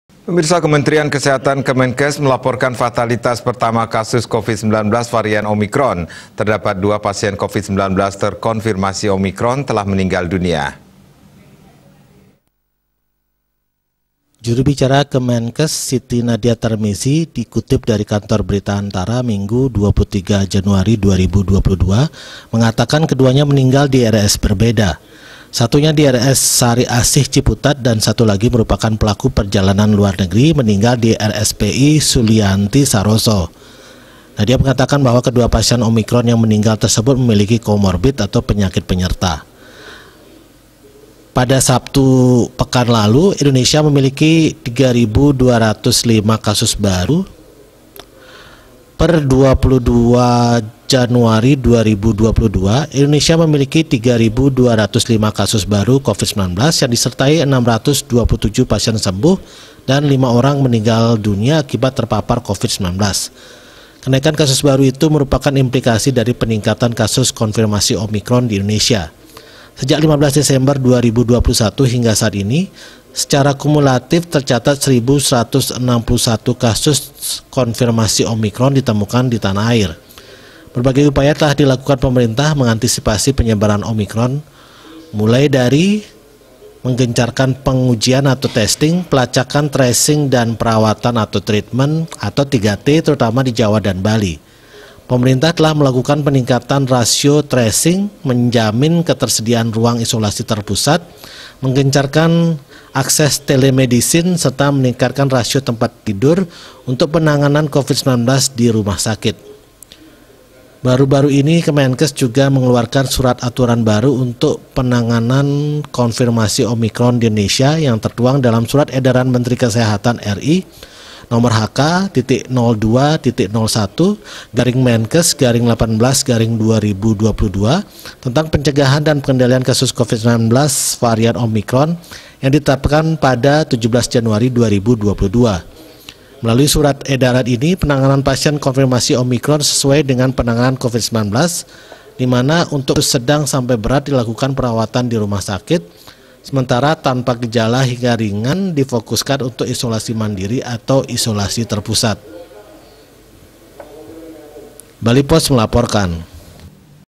KEMENKES LAPORKAN KEMATIAN PASIEN OMICRON
KEMENTERIAN KESEHATAN, KEMENKES MELAPORKAN FATALITAS PERTAMA KASUS COVID-19 VARIAN OMICRON. TERDAPAT DUA PASIEN COVID-19 TERKONFIRMASI OMICRON TELAH MENINGGAL DUNIA.